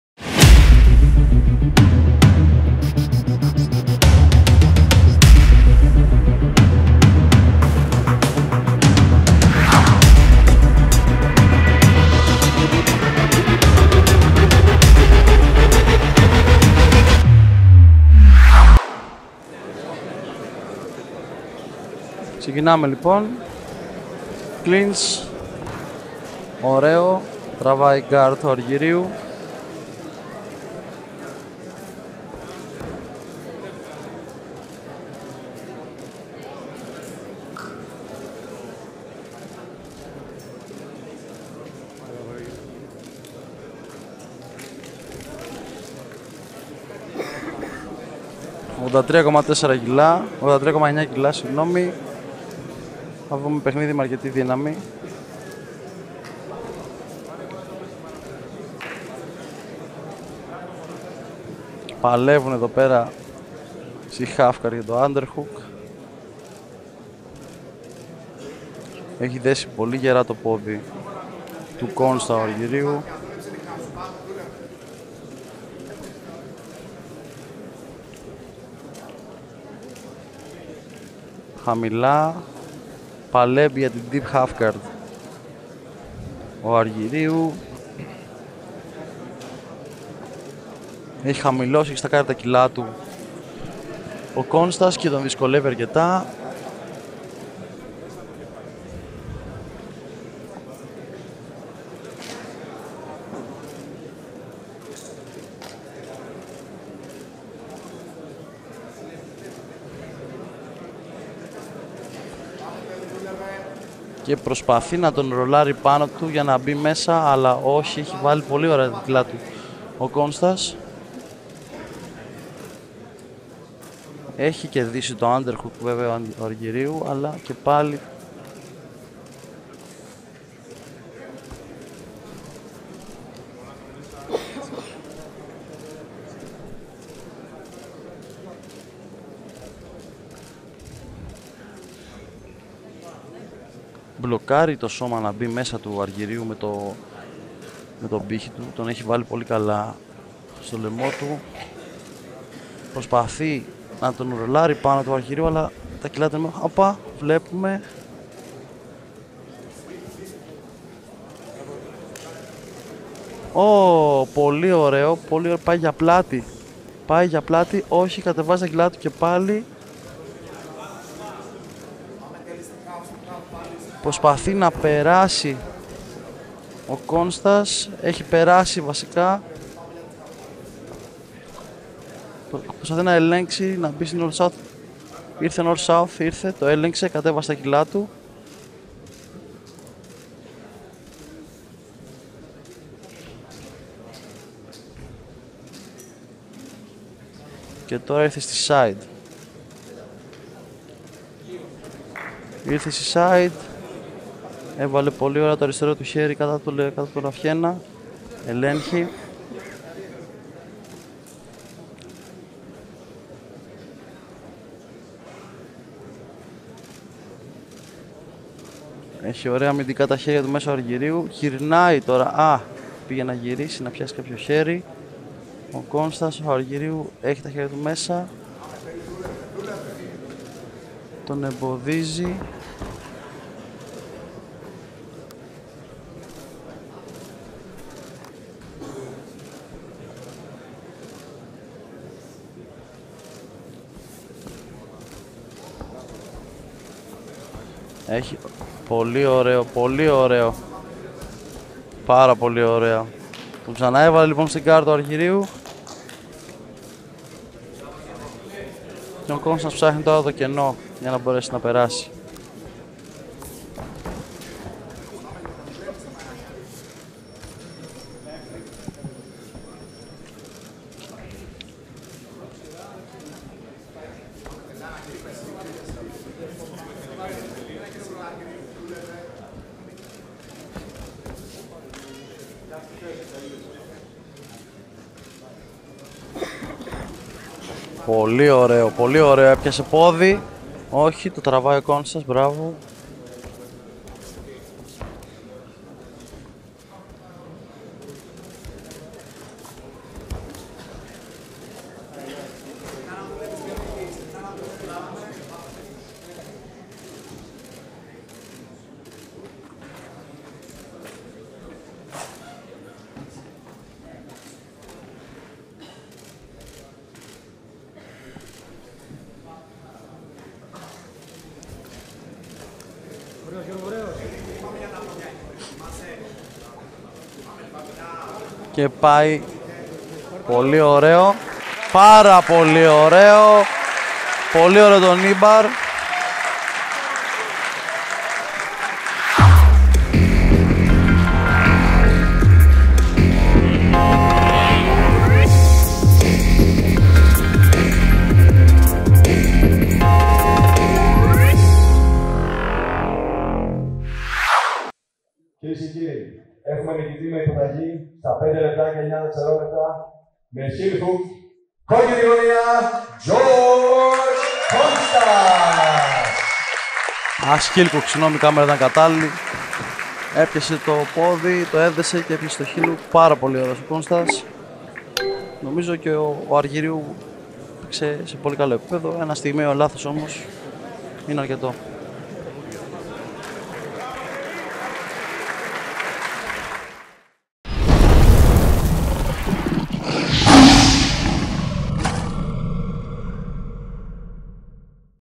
Submissionism 8: Tilemachos Argiriou vs Giorgos Konstas Full Fight
Name of Promotion: Cage Survivor
Name of Event: Submissionism 8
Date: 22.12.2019
Place: Arena the Core Sports Center
City: Athens
Country: Greece
Tilemachos Argiriou (0-0-0, Greece) vs. Giorgos Konstas (0-1-0, Greece)